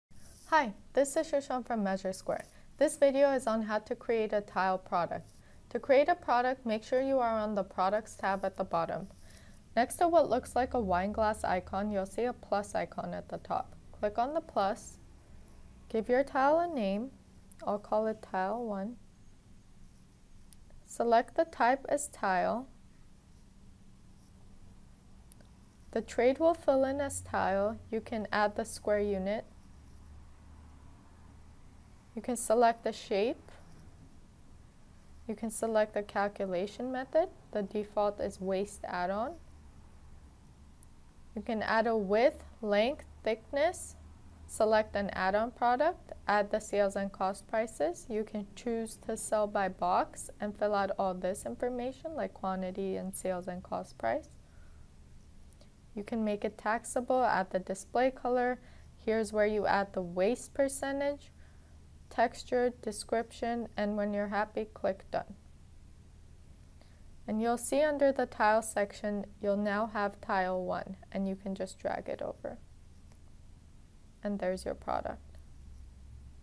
Tile - MeasureSquare iPad Edition
This video will show you how to create tile products.

The #1 Takeoff and Estimating Software MeasureSquare Mobile Edition.
Empower your sales rep to close sales onsite.